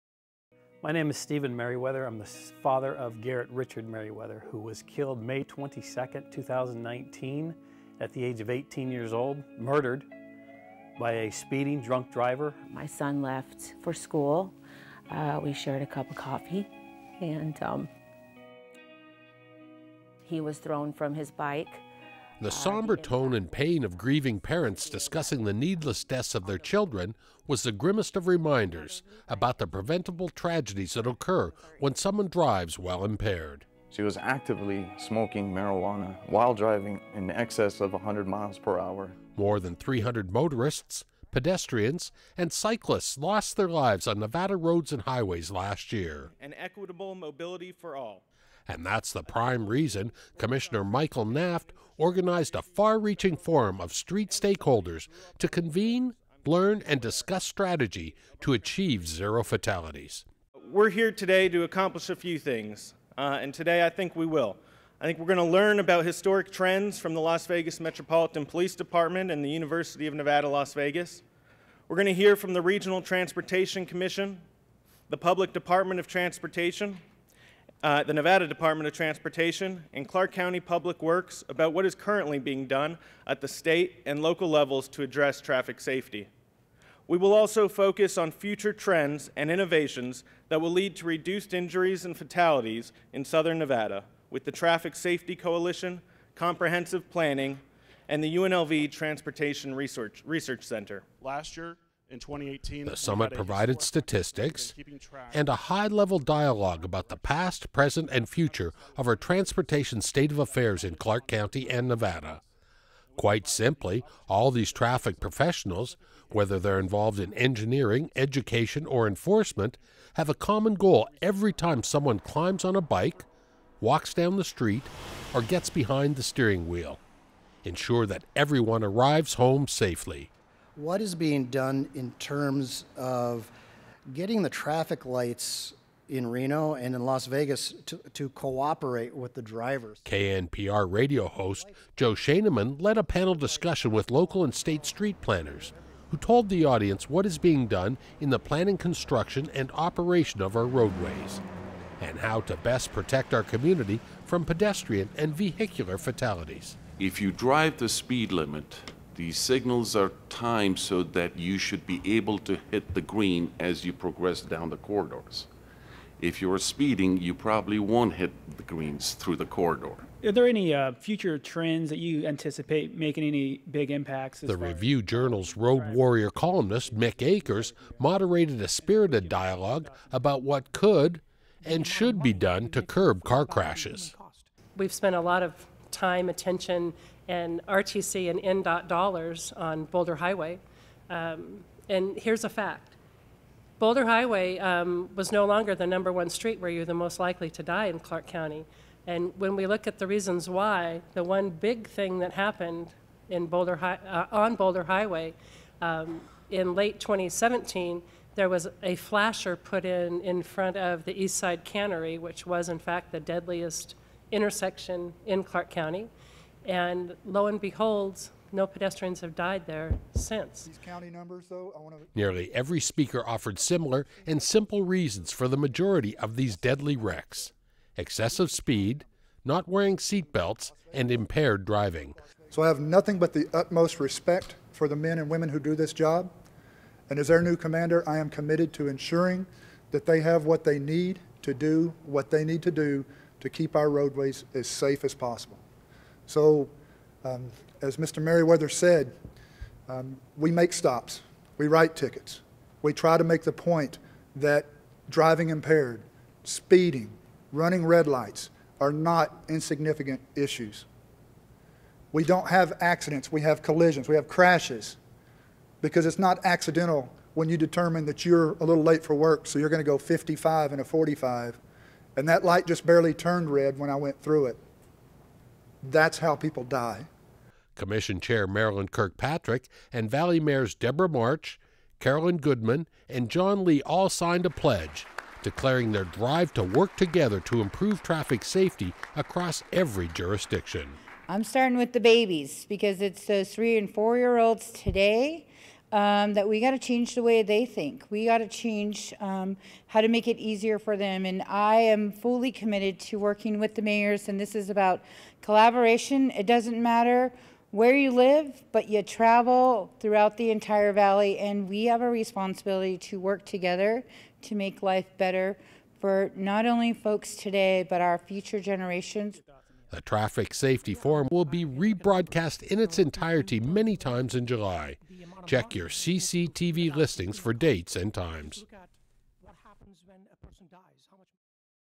Clark County Traffic Safety Forum Held at UNLV
With more than 300 traffic fatalities a year and the number of pedestrian deaths steadily rising, traffic safety was under the spotlight at a summit of traffic safety experts and Southern Nevada elected leaders recently at UNLV's Tam Alumni Center. The Traffic Safety Forum, organized by Clark County Commissioner Michael Naft, included representatives from the Las Vegas Metropolitan Police Department's Traffic Bureau, the Nevada Department of Public Safety, the Nevada Department of Transportation, the Clark County departments of Public Works and Comprehensive Planning, the Regional Transportation Commission, the Chairman of the County Commission, Marilyn Kirkpatrick, and the mayors of the cities of Las Vegas, Henderson, North Las Vegas and Boulder City. The forum featured a presentation with a historical overview followed by panel discussions of "What is being done?" and "What more can be done?" Another panel included a discussion by Chairman Kirkpatrick, Mayor Carolyn Goodman of Las Vegas, Mayor Debra March of Henderson, Mayor John Lee of North Las Vegas and Mayor Rod Woodbury of Boulder City. This last panel concluded with the signing of a pledge to work across jurisdictions to improve traffic safety.  Nevada traffic fatalities exceeded 300 the last four years (331 in 2018, 309 in 2017, 329 in 2016 and 326 in 2015), while serious injuries resulting from traffic crashes were similarly high with 1,102 in 2017, 1,273 in 2016 and 1,349 in 2015. Meanwhile, pedestrian fatalities have been trending upward over the last decade – from 56 in 2008 to a record high of 91 in 2017. The forum may be viewed on Clark County Television and is available for playing on-demand at YouTube.com/ClarkCountyNV.